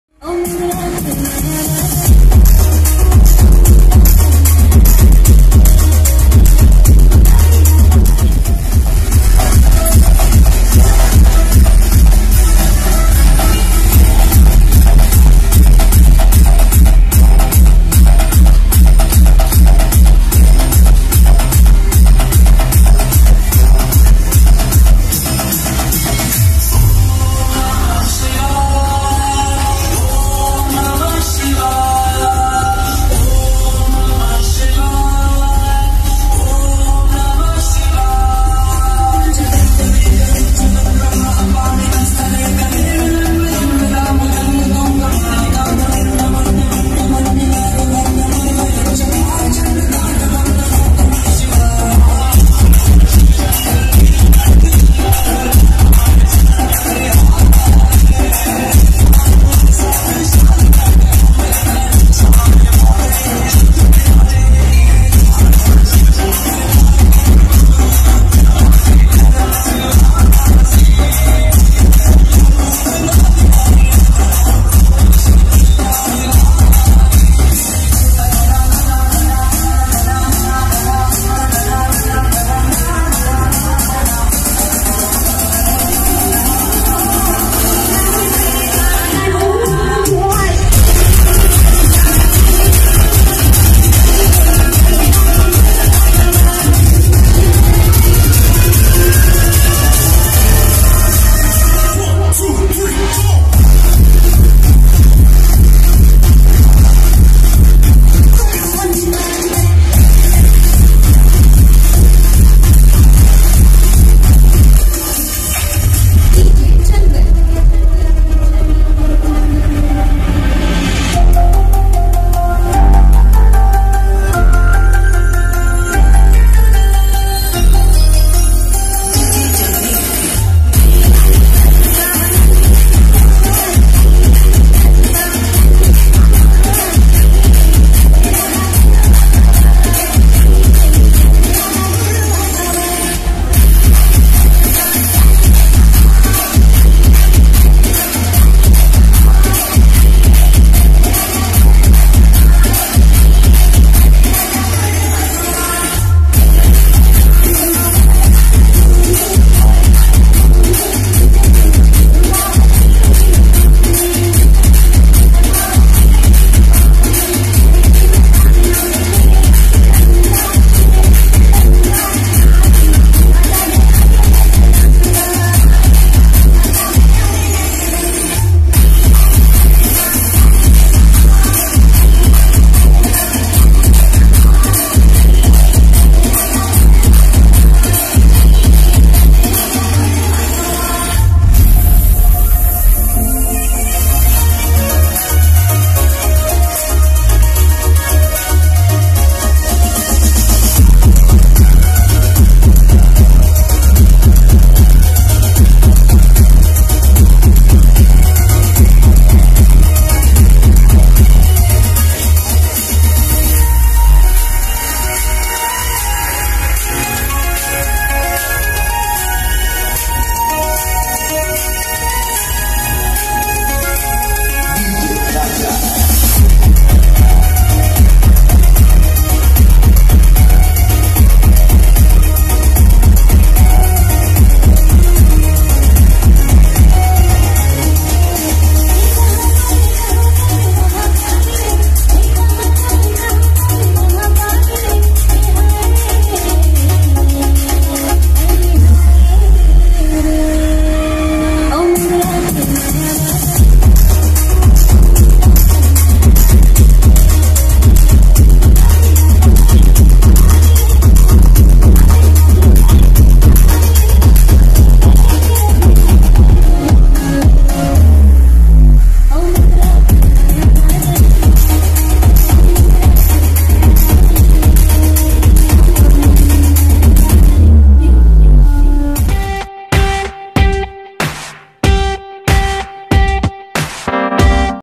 DJ SHEIKH | Chaattisgarhi Remix | HD Sound | CG04 LIVE
SHEIKH DJ ROADSHOW IN GANESH JHANKI RAIPUR 2023